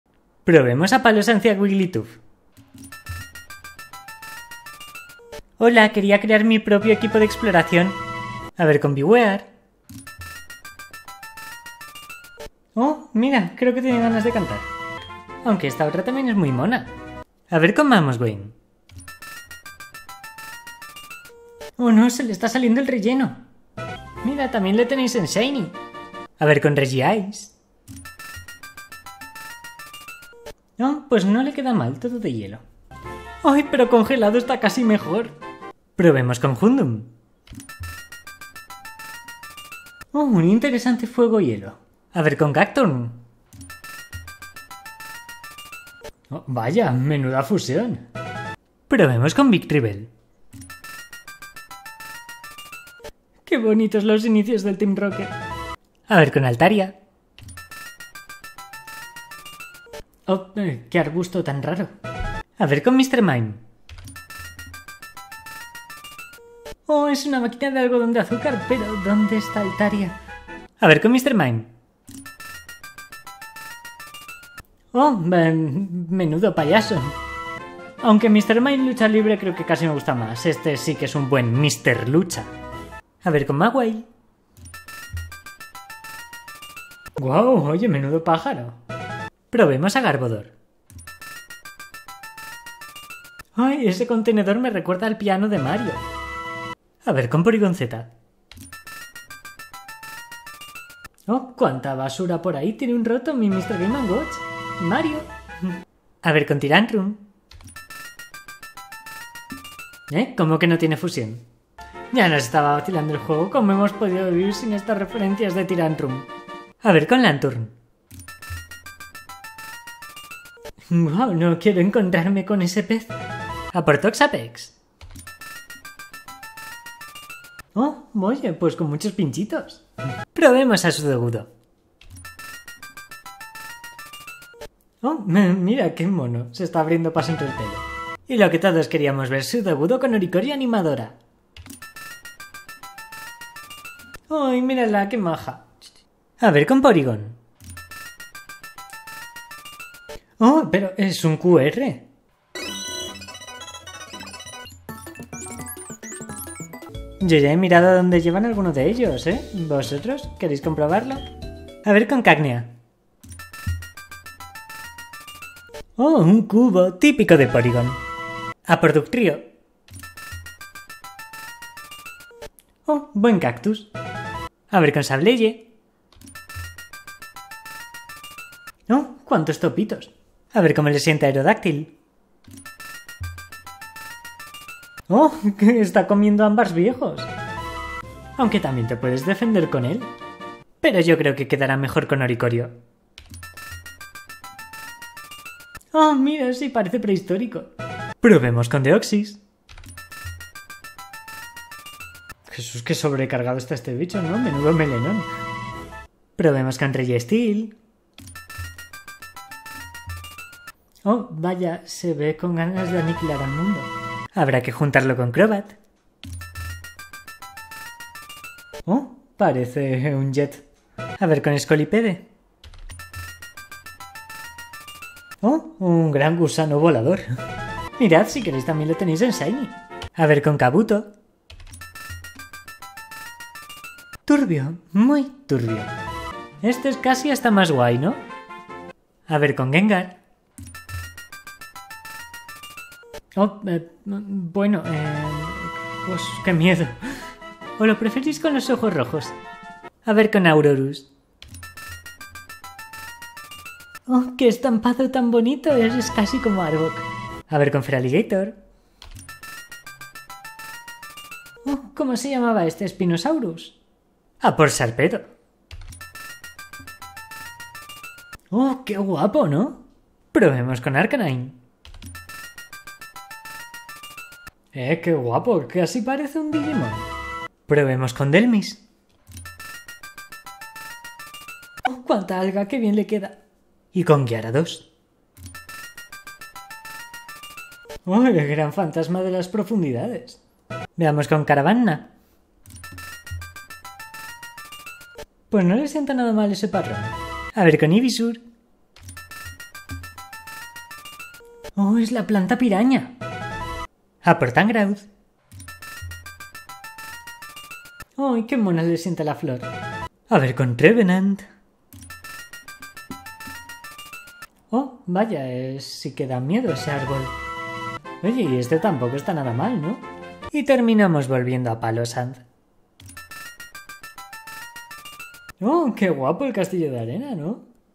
¡SELECCIÓN DE FUSIONES! (Parte 8)
00:00 Wigglytuff
00:08 Bewear
00:17 Mamoswine
00:26 Regice
00:35 Houndoom
00:42 Cacturne
00:48 Victreebell
00:56 Altaria
01:02 Mr. Mime
01:10 Hawlucha
01:23 Mawile
01:29 Garbodor
01:37 Porygon Z
01:46 Tyrantrum
01:56 Lanturn
02:03 Toxapex
02:10 Sudowoodo
02:18 Oricorio (Pom-Pom)
02:26 Porygon
02:37 Cacnea
02:44 Dugtrio
02:50 Sableye
02:56 Aerodactyl
03:05 Oricorio (Sensu)
03:13 Deoxys
03:21 Registeel
03:29 Crobat
03:36 Scolipede
03:45 Kabuto
03:54 Gengar
04:05 Aurorus
04:12 Feraligatr
04:19 Sharpedo
04:25 Arcanine
04:33 Dhelmise
04:39 Gyarados
04:46 Caravanha
04:53 Ivysaur
04:59 Tangrowth
05:05 Trevenant
05:16 Palossand